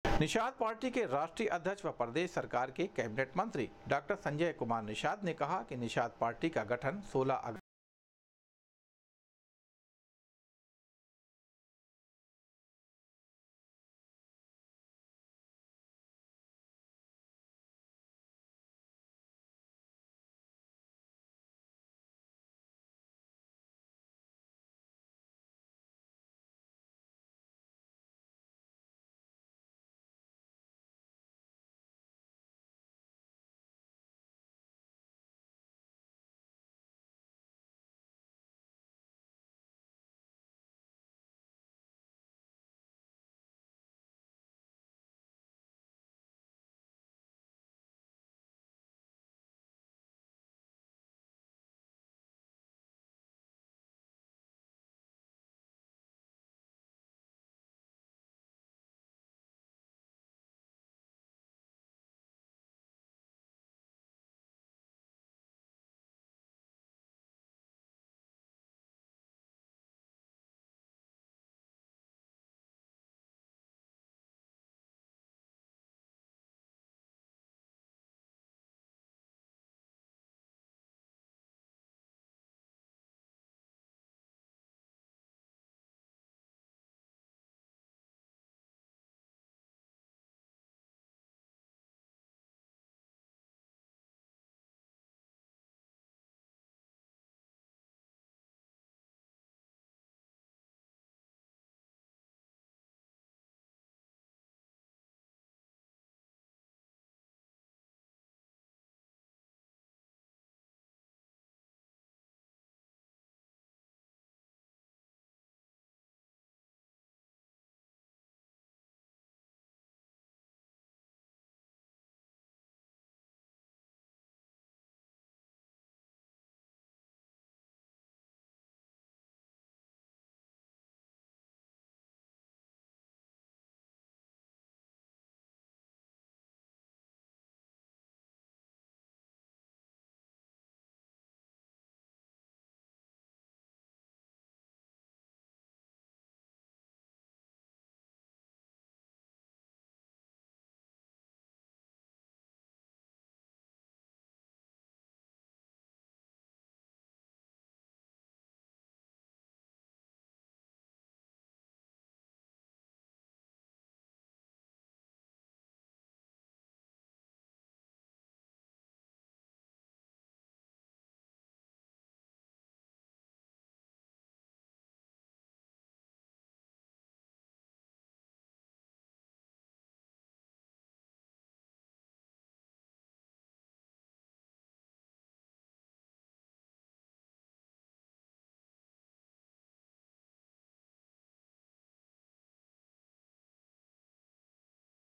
सोलह अगस्त को महायोगी मत्स्येंद्रनाथ की धरती पर निषाद पार्टी का मनाया जाएगा आठवां स्थापना दिवस
सोलह अगस्त को महायोगी मत्स्येंद्रनाथ की धरती पर निषाद पार्टी का मनाया जाएगा आठवां स्थापना दिवस,आगामी लोकसभा चुनाव अपने सिंबल पर लड़ेगी निषाद पार्टी,प्रदेश सरकार की कैबिनेट मंत्री व निषाद पार्टी के राष्ट्रीय अध्यक्ष डॉ0 संजय निषाद ने दी जानकारी